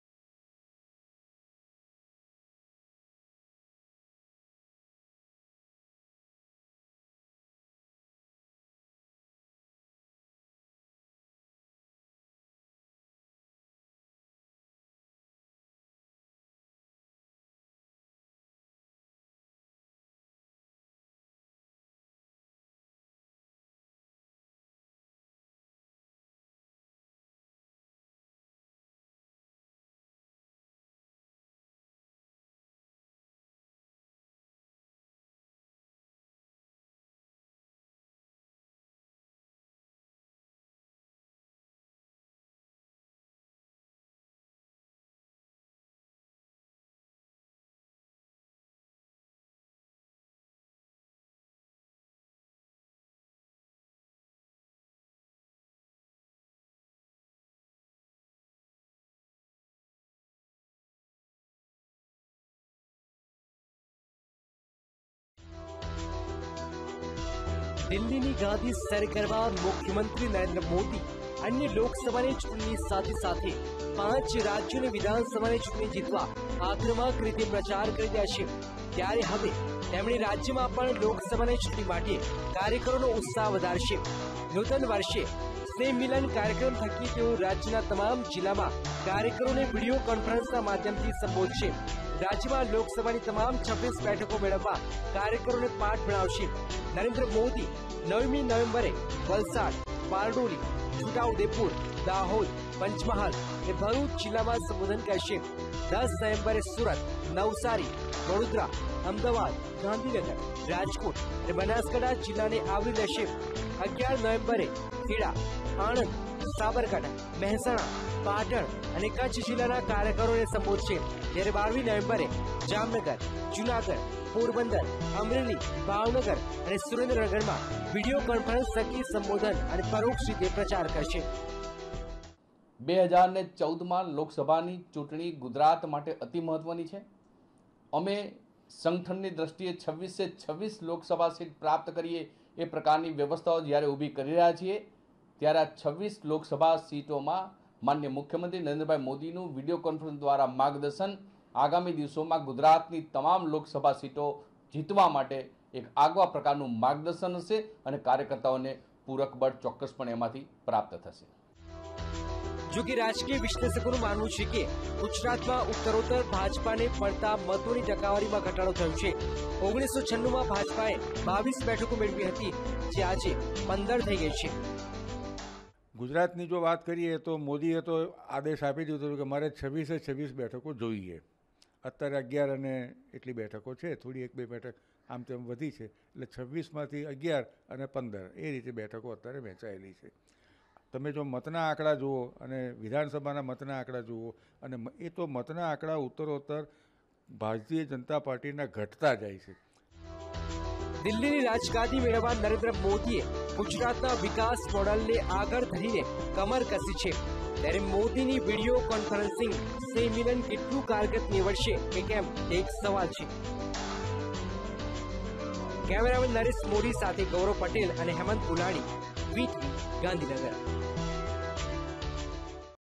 Chief Minister Modi to campaign for seats in the Lok Sabha - vtv
મુખ્યમંત્રી નરેન્દ્ર મોદી આજથી 12 નવેમ્બર સુધી રાજ્યમાં નૂતનવર્ષનિમિત્તે સ્નેહમિલન સંમેલન થકી  લોકસભાની બેઠકો માટે પ્રચાર કરશે. તેઓ ચાર વિભાગોમાં રાજ્યના તમામ જિલ્લાઓમાં વીડિયો કોન્ફરન્સથી કાર્યકરોને સંબોધન કરશે.